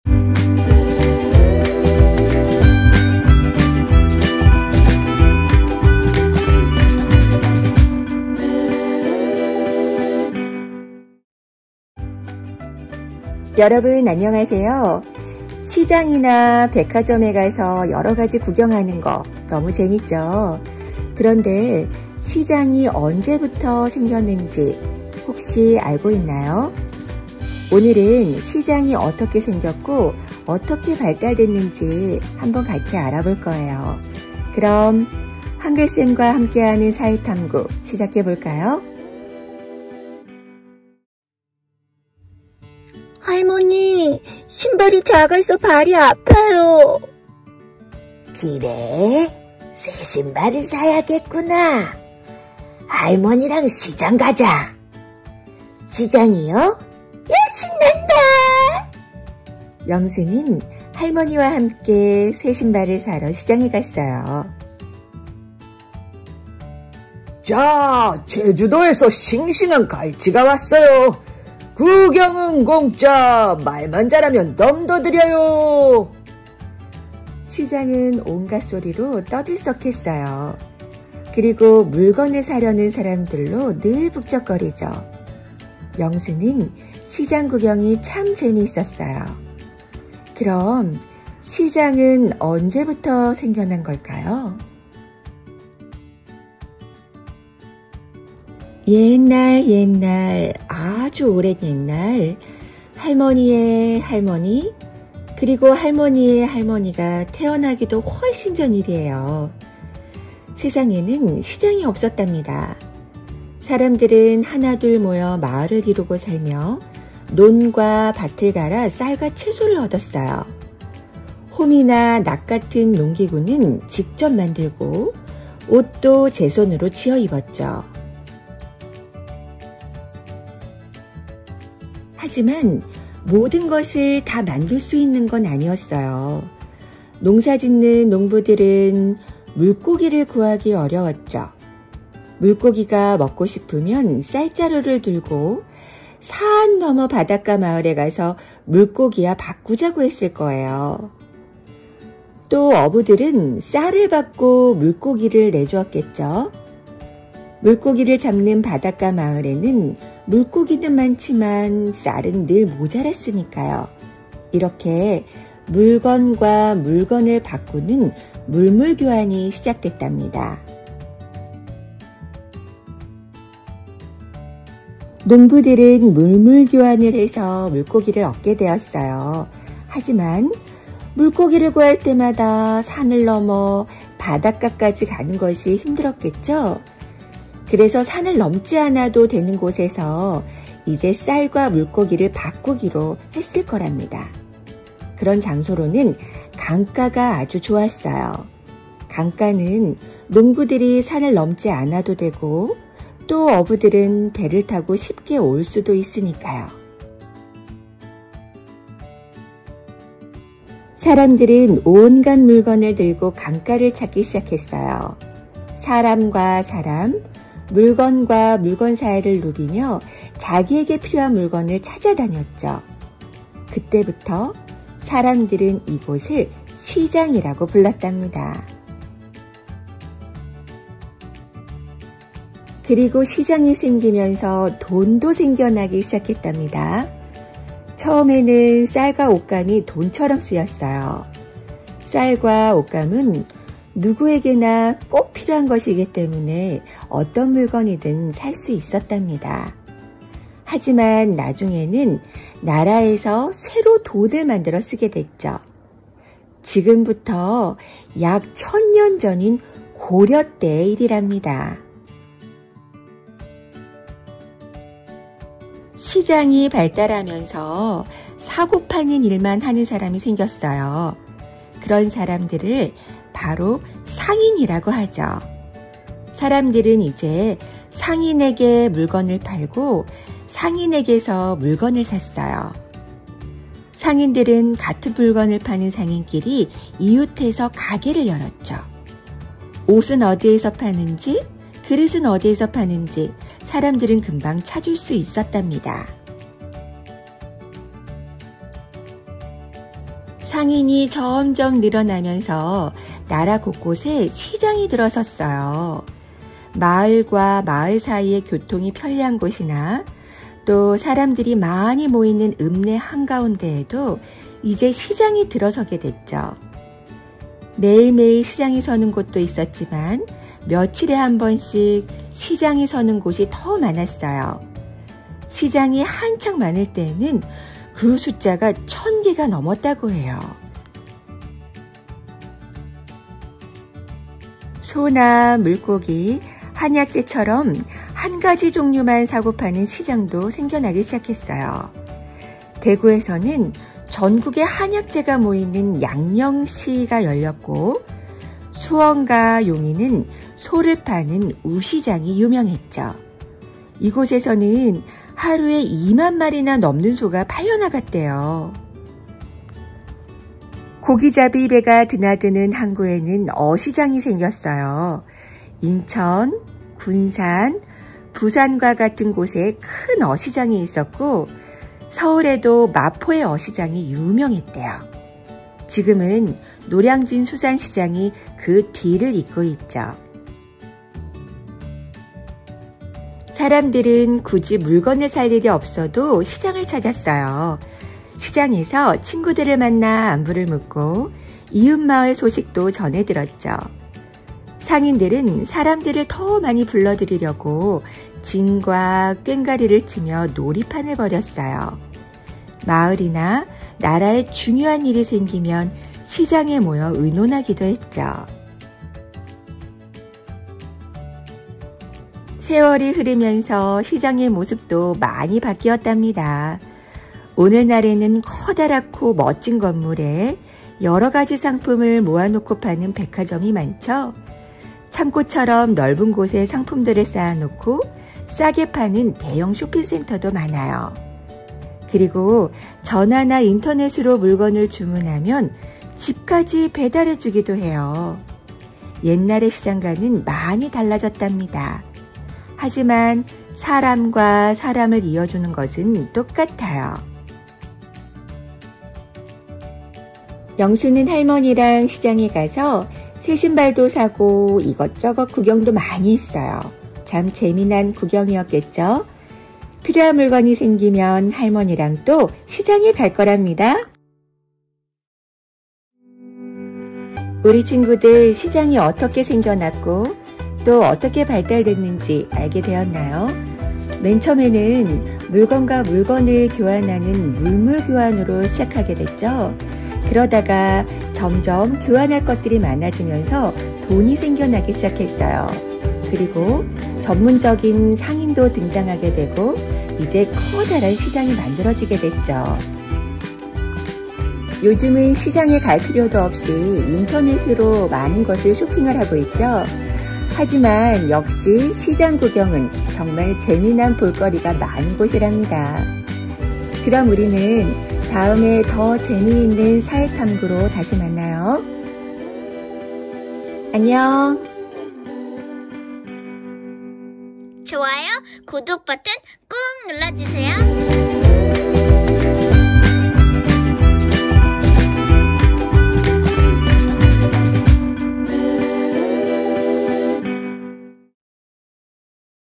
[한글쌤과 함께하는]사회탐구-시장은 어떻게 생겨났을까? |동화책읽어주기|지식동화 |교육동화
[한글쌤과 함께하는] 사회탐구 - 시장은 어떻게 생겨났을까?
시끌시끌 시장구경
시장이 어떻게 생겨나게 되었고, 어떻게 발전하게 되었는지 함께 알아봐요.
신나는 동화여행 속으로 출발!

*도서정보
글: 정재윤
그림:이육남
출판사: 한국노벨
본 동화는 출판사에서 더 이상 판매되지 않는 절판된 동화입니다.

한글자막이 필요하신 분은, 자막설정을 켜고 들어주세요.
감사합니다.